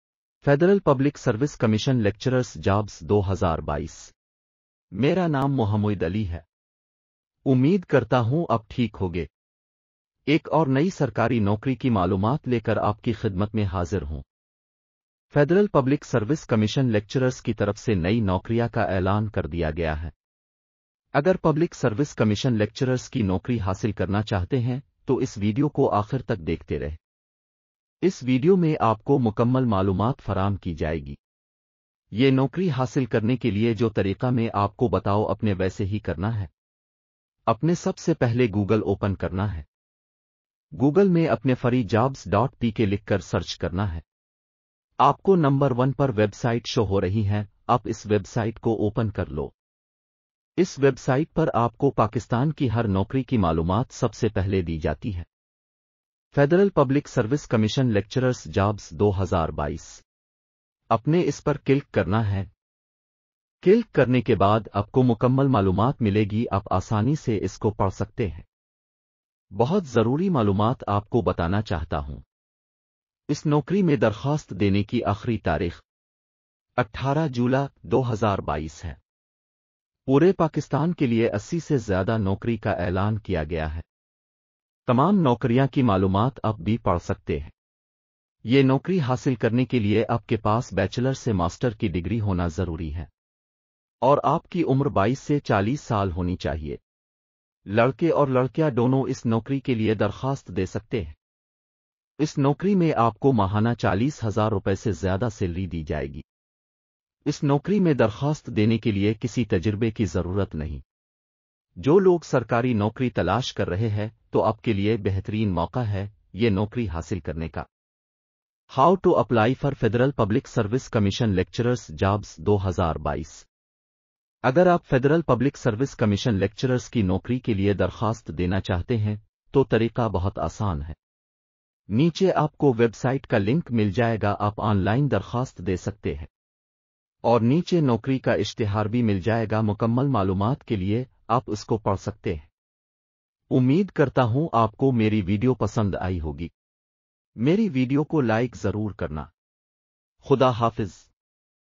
Lecturers Jobs 2022 Latest (Male/Female)
FPSC Lecturers Jobs 2022: Federal Government Educational Institutions (Cantt & Garrison Colleges) are hiring applications for the Federal Public Service Commission through the Ministry of Defence Government of Pakistan.

The Federal Public Service Commission advertises jobs for FGEI lecturers in Cantt and garrison colleges. These jobs are listed in the FPSC Jobs Listing. Interested applicants looking for Government Jobs should not miss this employment opportunity.